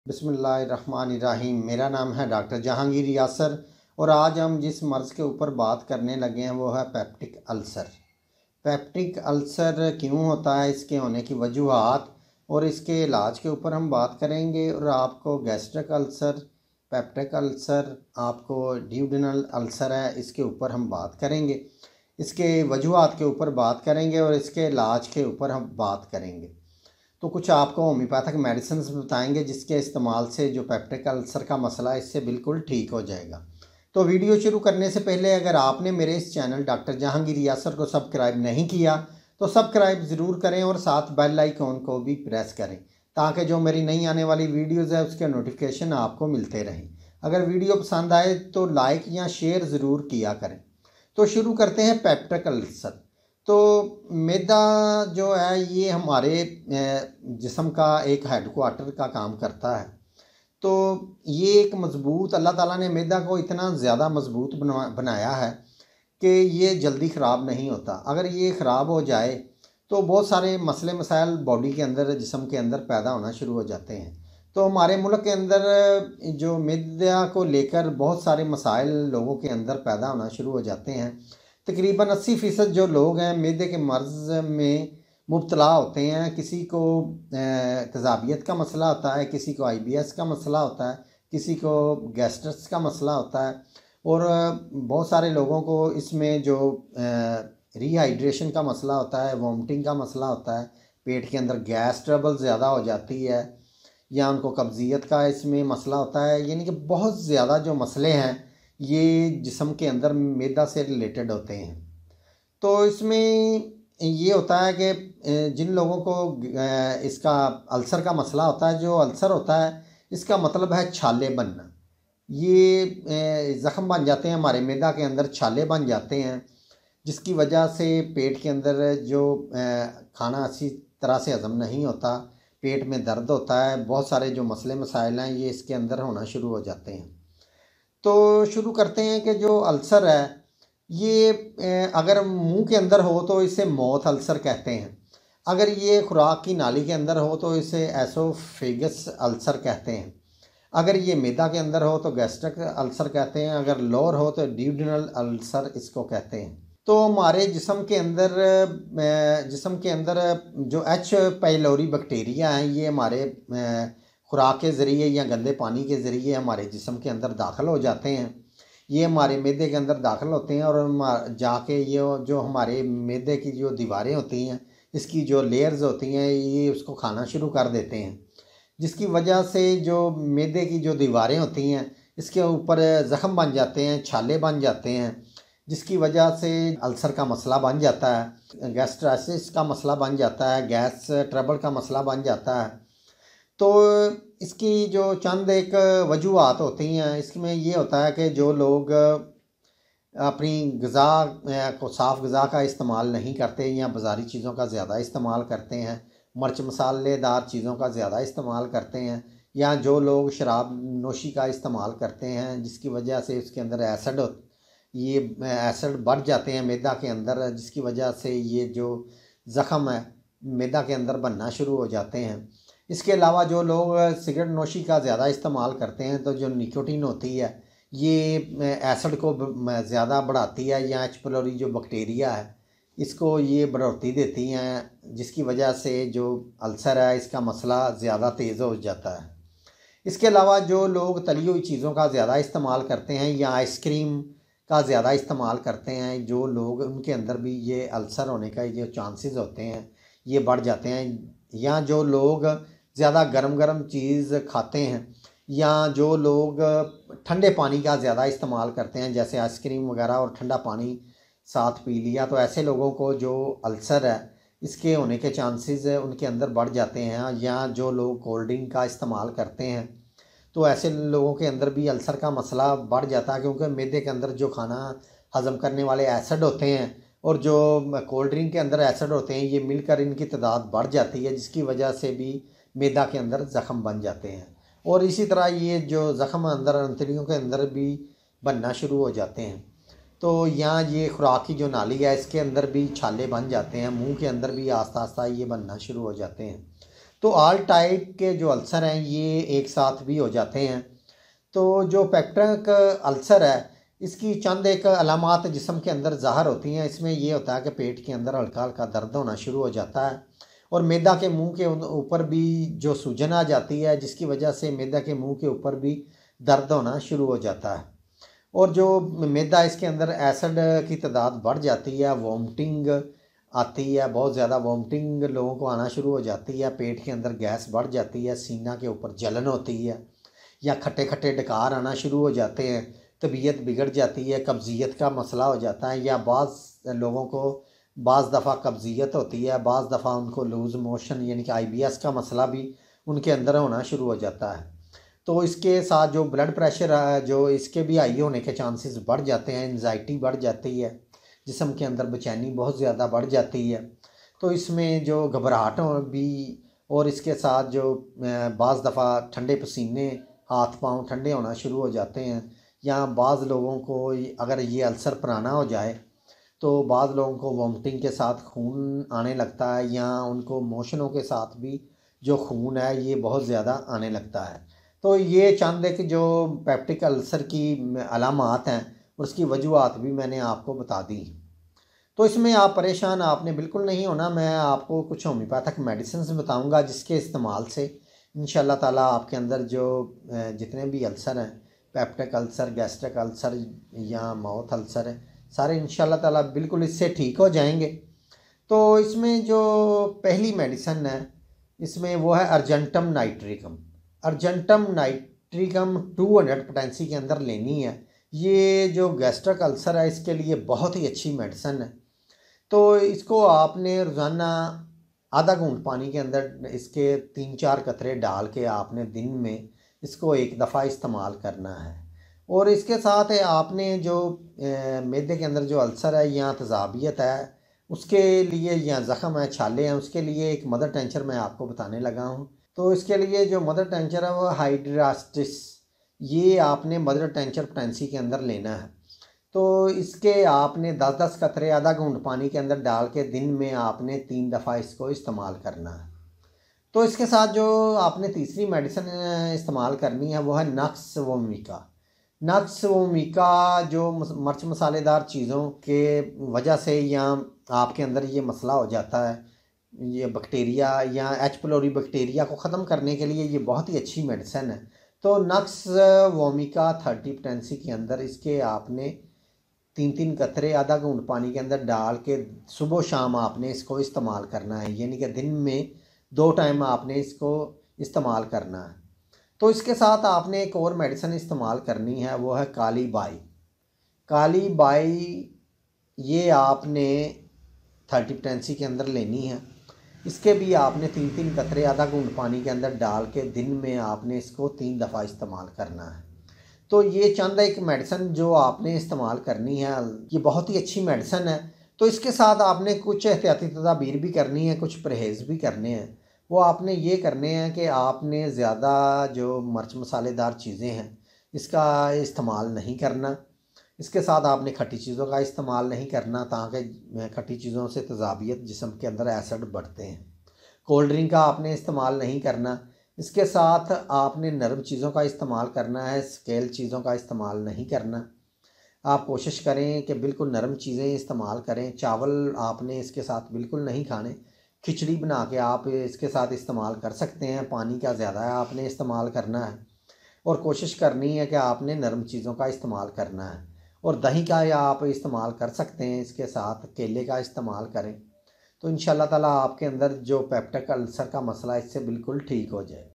Peptic ulcer treatment in urdu | Stomach ulcer ka ilaj By Dr Jahangir Yasir | Gastric ulcer
Welcome to Dr Jahangir Yasir Channel

Today Dr Jahangir Yasir is discuss in this video about Peptic ulcer Disease treatment in urdu,symptoms,reasons and Precautions.

Homeopathic Medicine:

1- Argentum nitricum 200
2- Hydrastis Q
3- Nux Vomica 30
4- Kali Bich 30

Mix 3 drops of Argentum nitricum 200 in water, 1 time a day.

Mix 10 drops of Hydrastis Q in water, 3 times a day.

Mix 3 drops of Nux Vomica 30 in water, 2 times a day.

Mix 3 drops of Kali Bich 30 in water, 3 times a day.

Duration:

Minimum 1 month use.